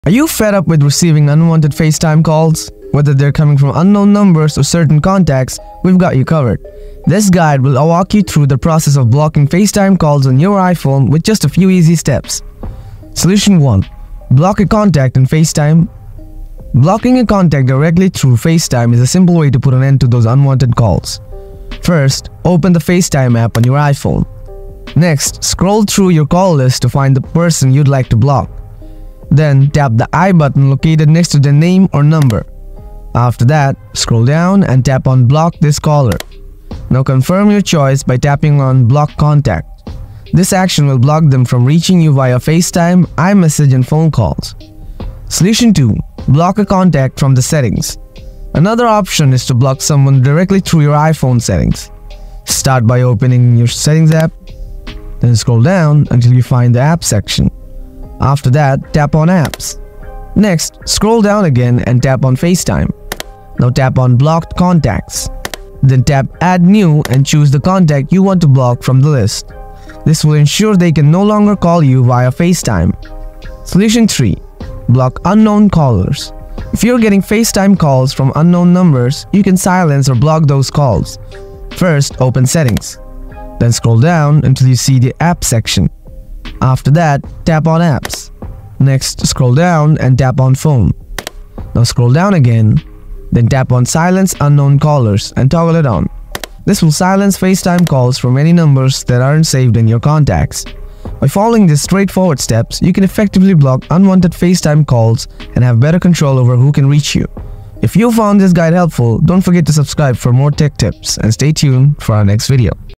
How To Block FaceTime Calls on iPhone | Block Anyone on Facetime Using Your iPhone!
Looking to block unknown callers on FaceTime on your iPhone or do you simply want to stop getting calls from a specific unknown caller on FaceTime? Well, either way, following this video will help you block any kind of spam calls on your Facetime on your iPhone and maintain your privacy! Those random calls can get pretty annoying and after a while, it becomes absolutely necessary to block the fake calls on Facetime if you want some peace in life!

So let's not waste your time and get into blocking the unwanted Facetime calls on your iPhone!

00:01- Video Intro
00:13- The Tutorial
01:47- Tutorial End